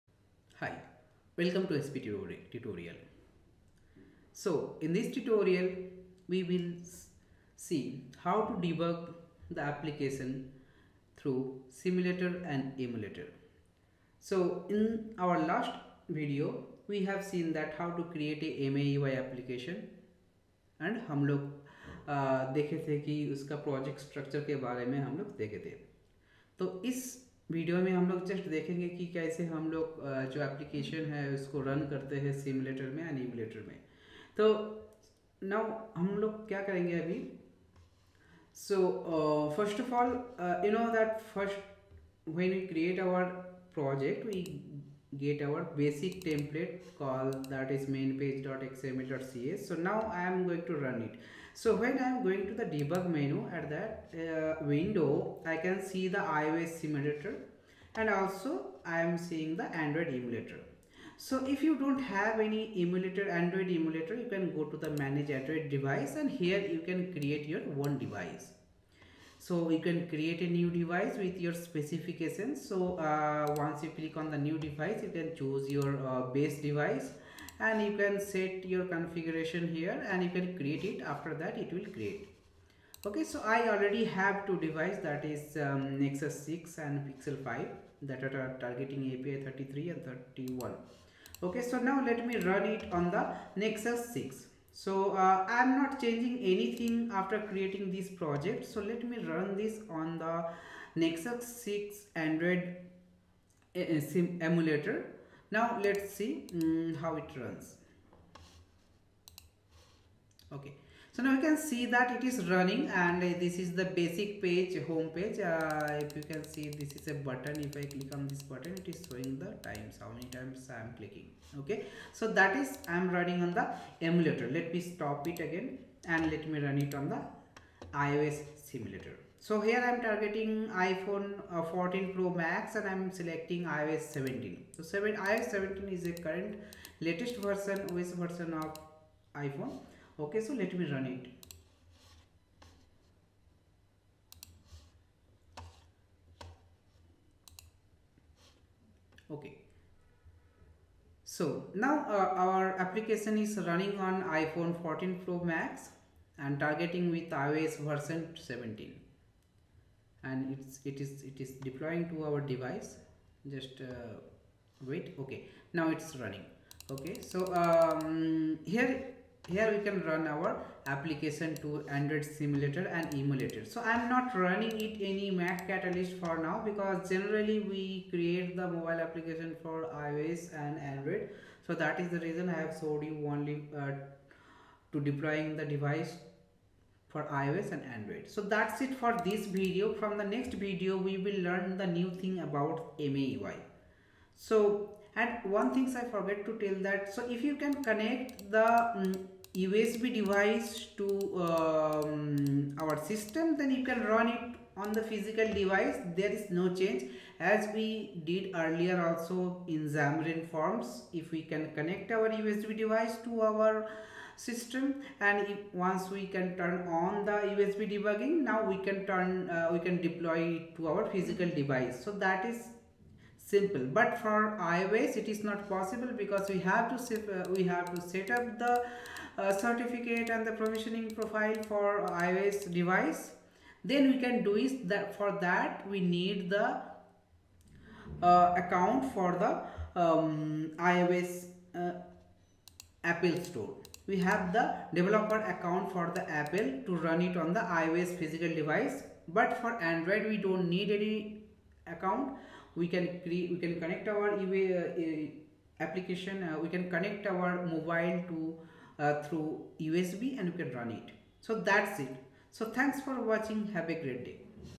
Debugging MAUI Application - .Net MAUI Tutorial
.Net MAUI Tutorial
In this video I shown, How to debug the app to iOS Simulator and Android Emulator.